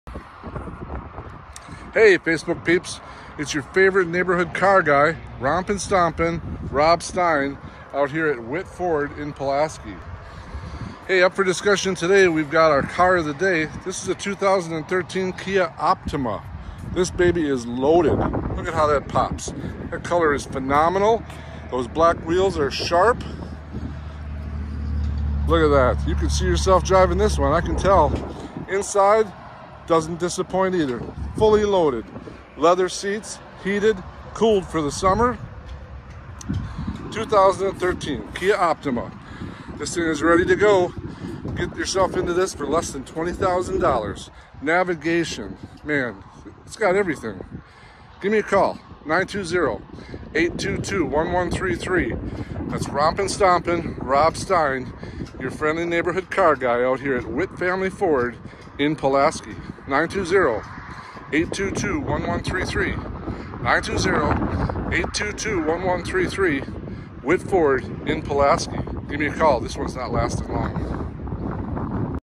Deal of the day for May 20, 2022 Rob @ Pulaski
2013 Kia Optima
96,000 miles
This car pops!!! This just hit the lot TODAY!!!
special price! Call and find it out!!!
Fully loaded, navigation, heated and cooled seats, big screen!
Rompin Stompin Rob Stein
Witt Family Ford
Pulaski WI